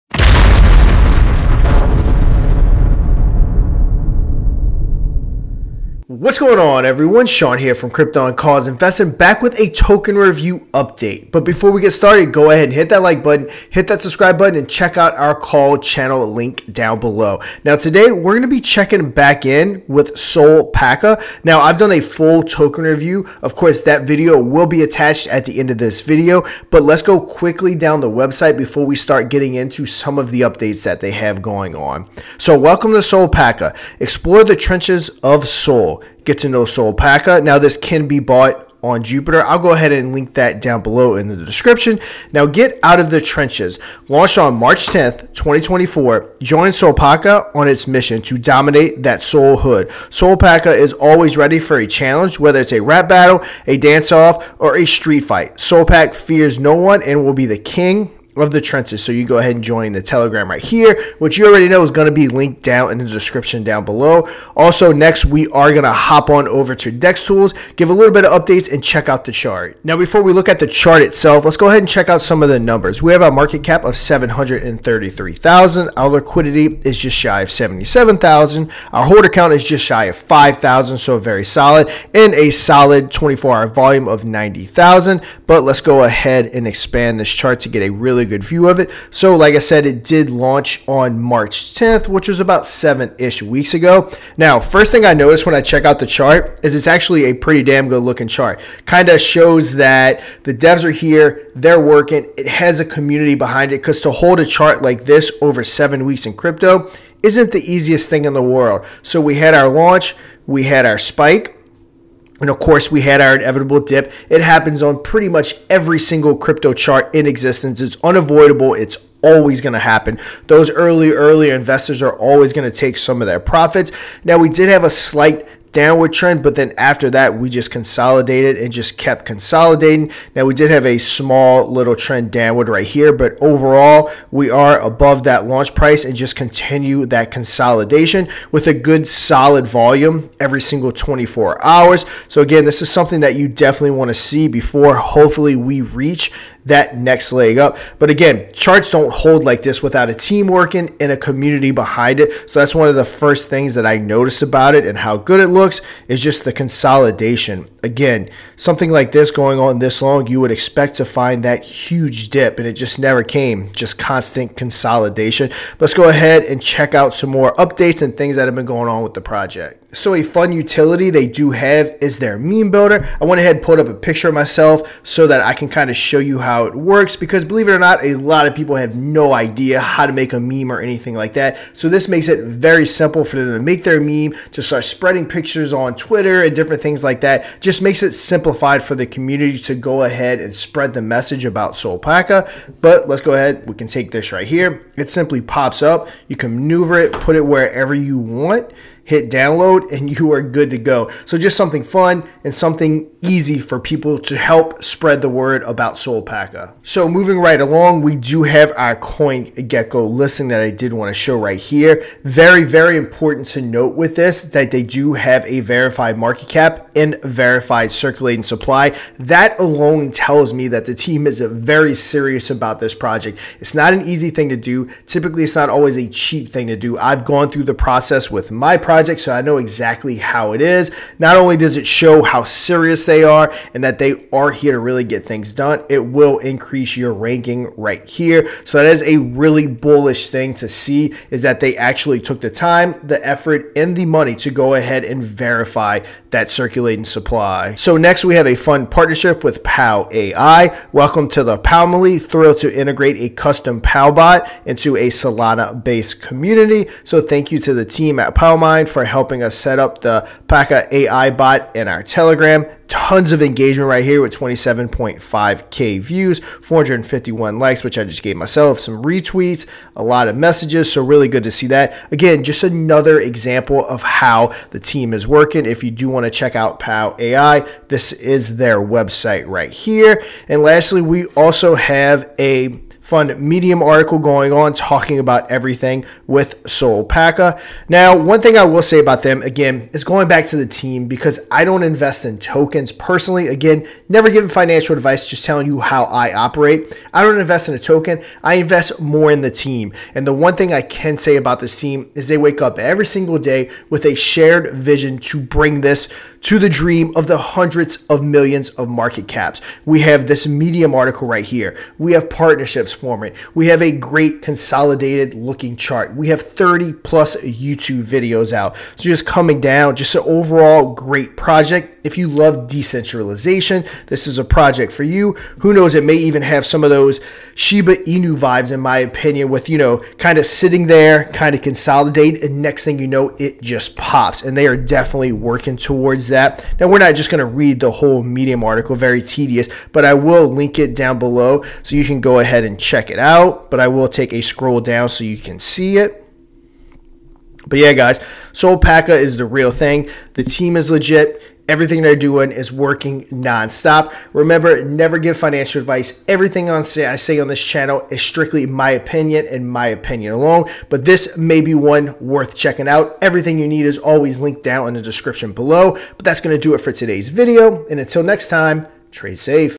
🚀Solpaca🚀Update🚀Huge Community🚀Based Devs🚀
Solpaca is looking very good. Based community and devs. Verified supply on Coingecko, 30+ Youtube videos, meme tool, and so much more going on. I genuinely believe this one may be a diamond in the rough.

NEVER purchase a token off of my review alone. ALWAYS DYOR and decide if its right for you.